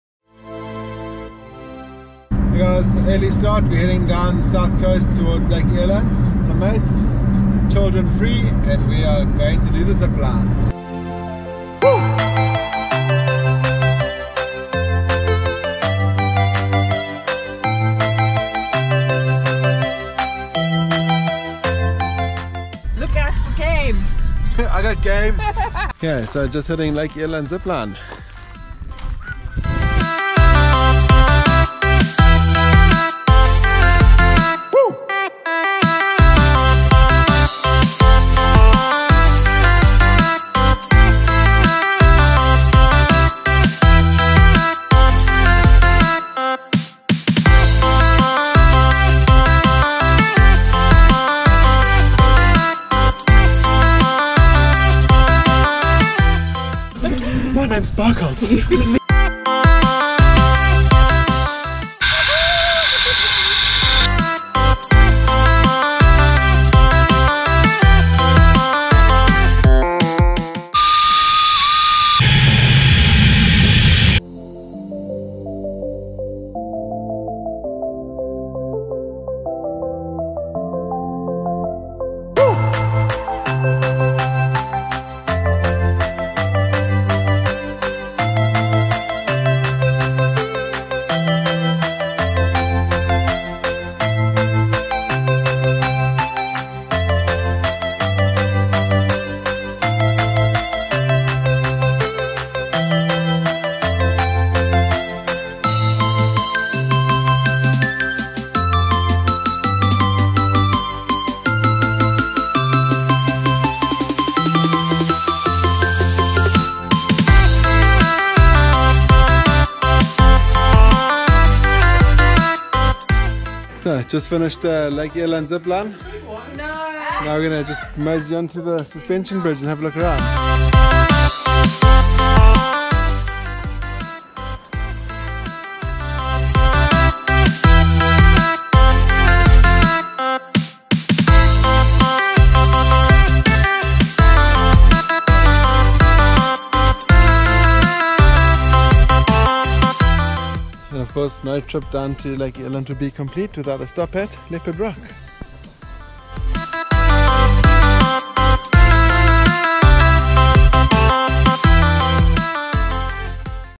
LONGEST ZIPLINE IN AFRICA - EXPLORING KZN
Incredible day trip down the coast of Kwa-Zulu Natal, South Africa, to an amazing adventure at Lake Eland. 4.5km of surreal surroundings while zipping around the lush green gorge!! Definitely still our favourite Zipline we've ever done!